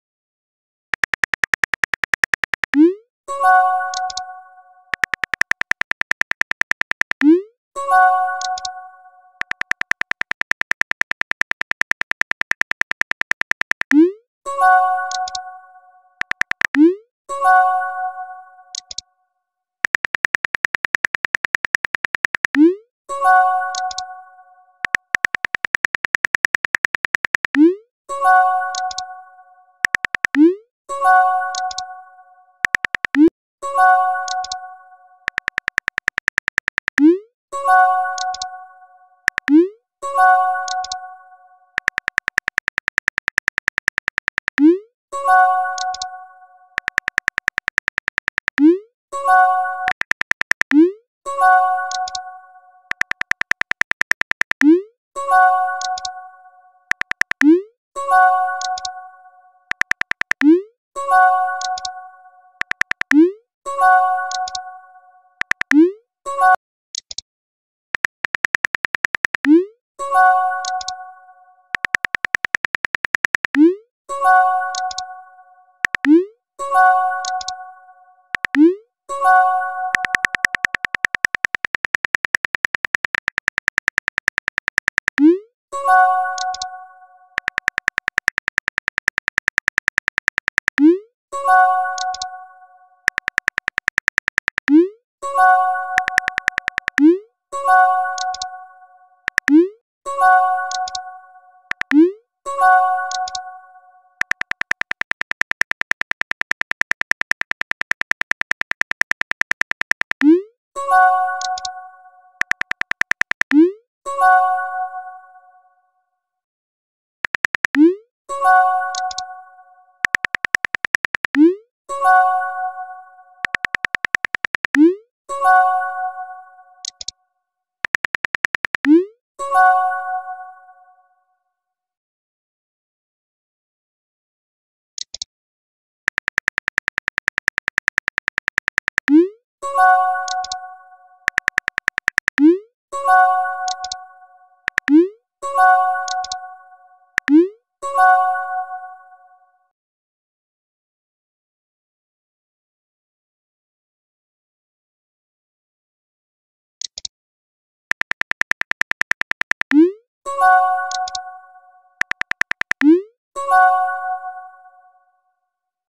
The Rap Game : L&N 🫶🏻✨😍
Theyre From Rap Game Season 2. and no these are not charecters from the story I like The Rap game and i watch it so im just doing a texting story bout them lol